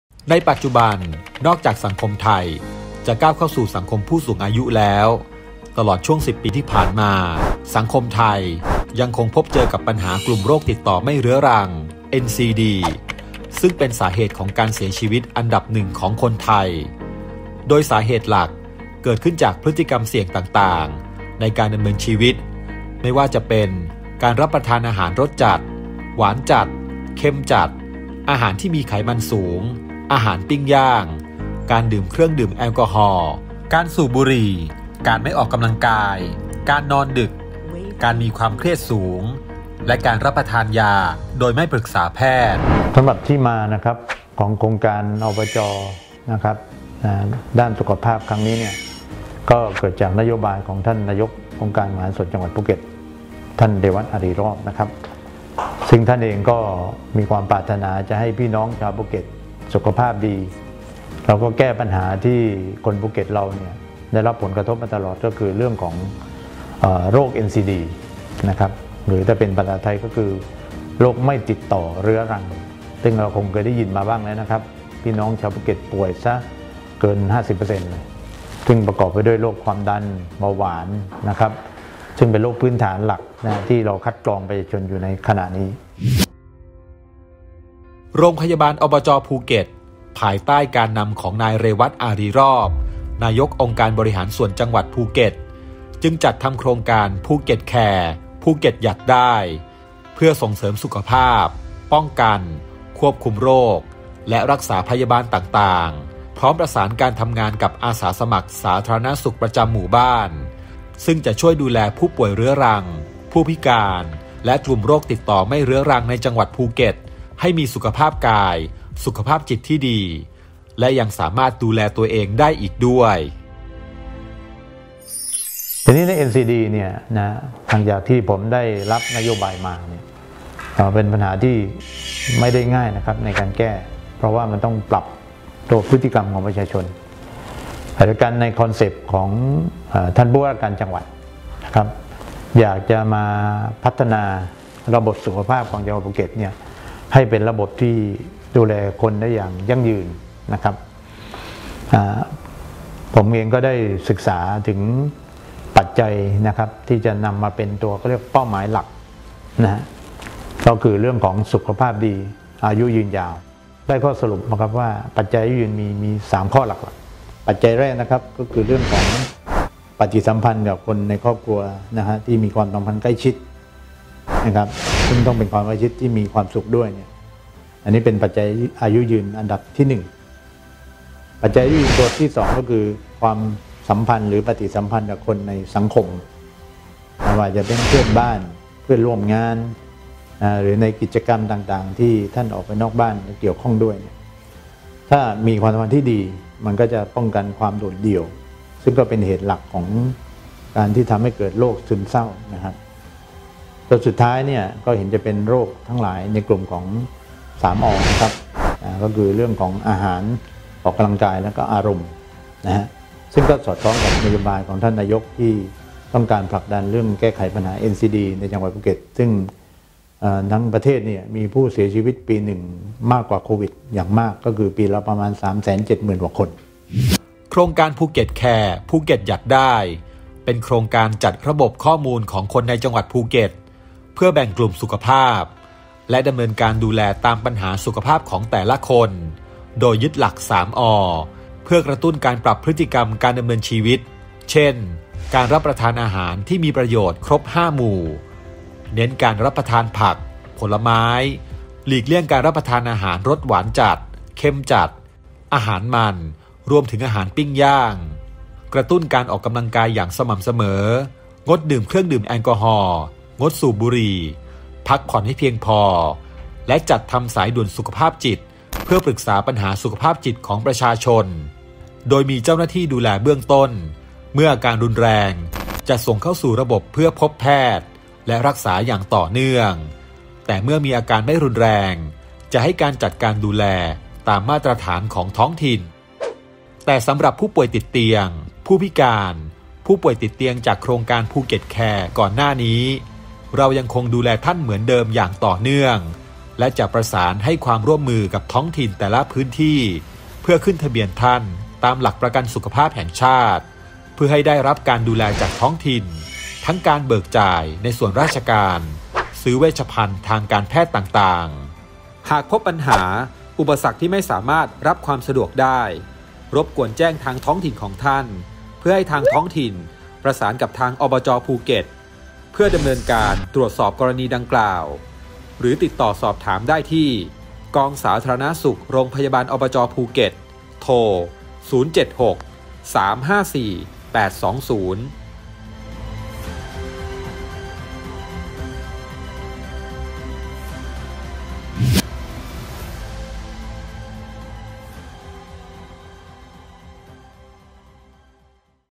Healthy Tips EP.2 (เดือนมกราคม 2565) “ภูเก็ตแคร์_ภูเก็ตหยัดได้”
👩🏻‍⚕️ “ภูเก็ตแคร์_ภูเก็ตหยัดได้”
เป็นโครงการที่ต่อยอดจาก”โครงการภูเก็ตแคร์” คือจะป้องกันไม่ให้คนภูเก็ตเป็นโรคติดต่อไม่เรื้อรัง เน้นเรื่อง3 อ คือ อาหาร , ออกกำลังกาย และ อารมณ์ดี 🏃🏃🏿‍♂️🏃‍♀️

🏥โรงพยาบาล_อบจ_ภูเก็ต (ประจำเดือน มกราคม 2565 EP.2)